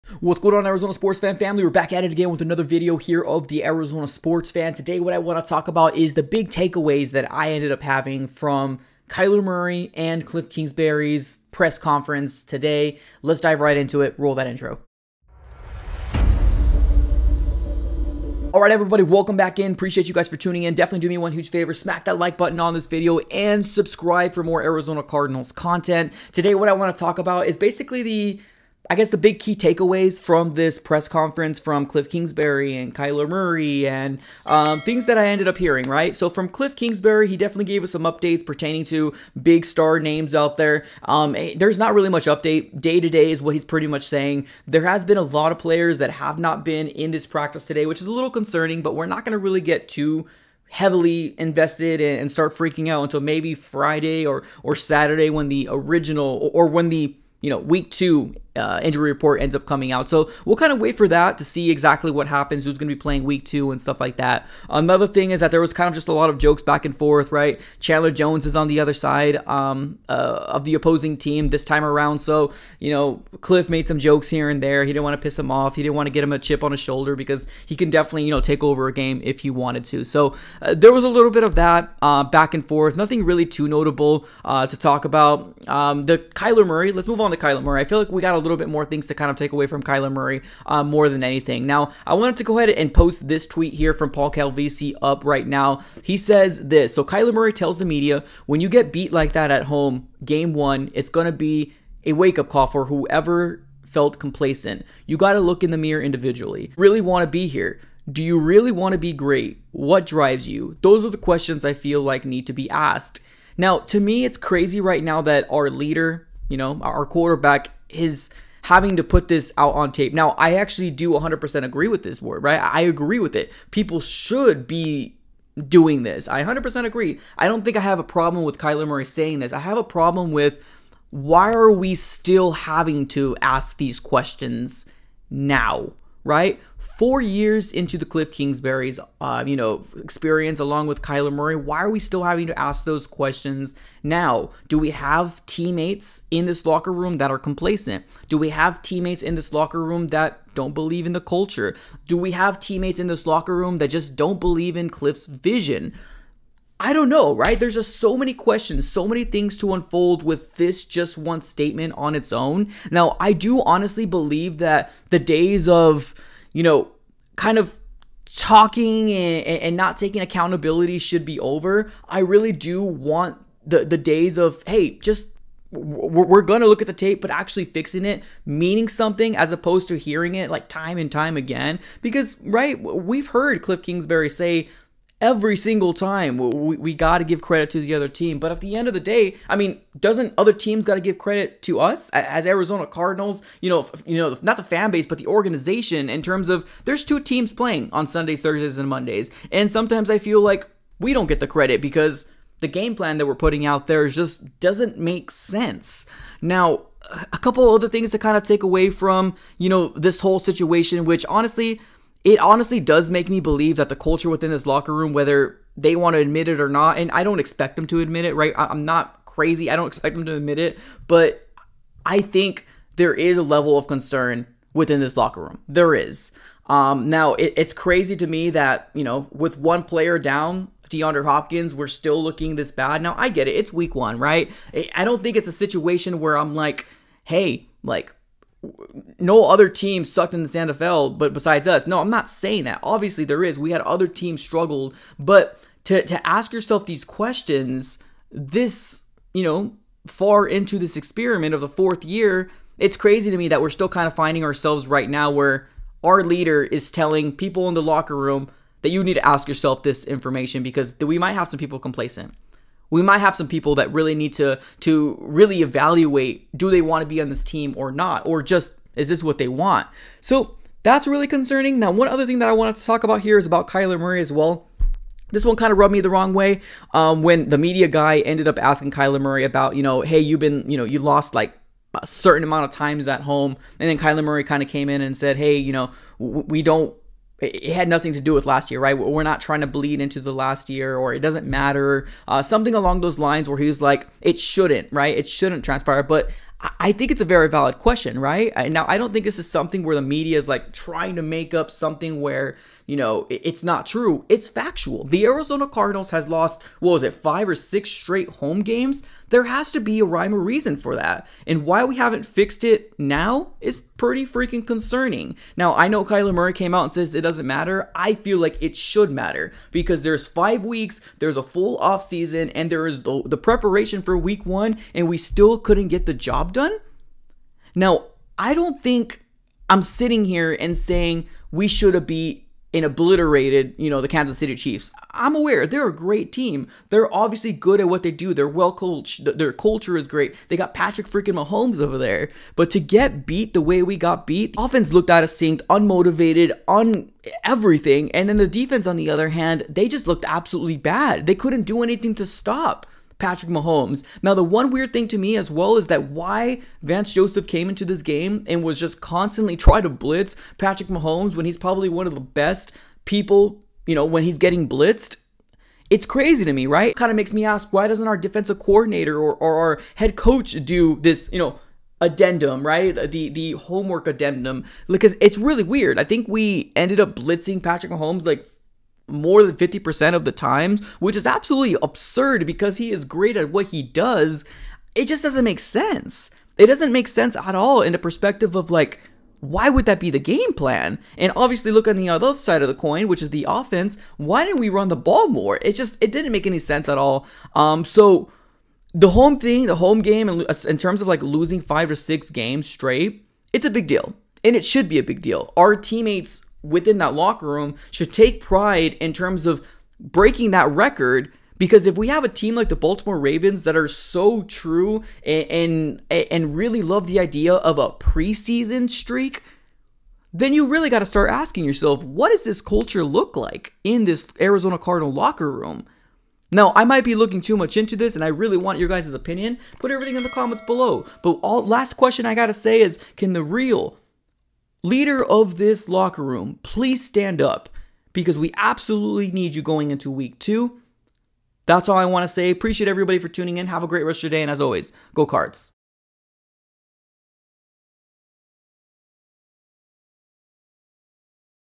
Big Key Takeaways From Todays Press Conference | Will The Real Leader Please Stand Up!?
Big Key takeways from today Press Conference from Kyler Murray and Kliff Kingsbury.

Arizona Cardinals 2022 Training Camp has finally arrived !!!